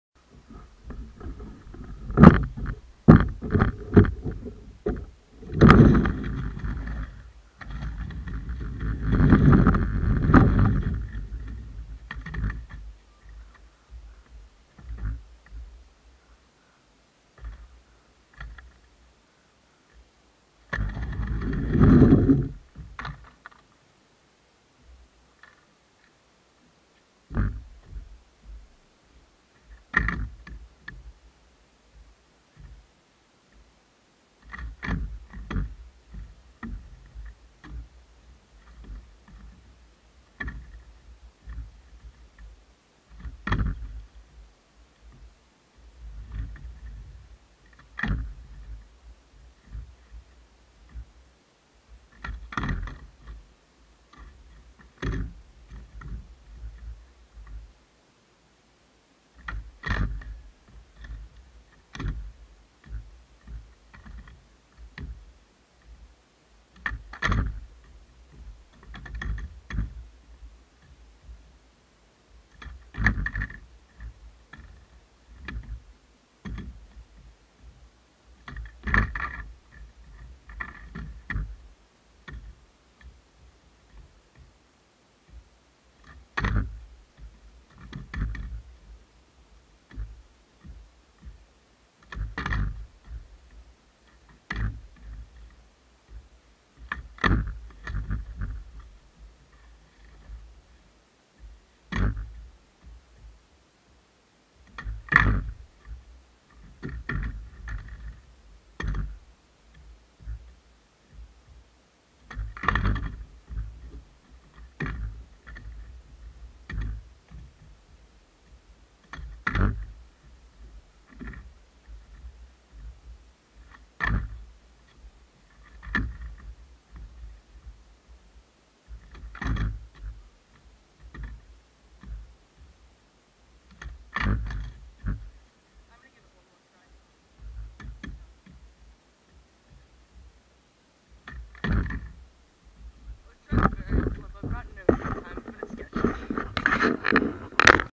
mini ramp sesion
tried to land verial flip at end of video but couldn't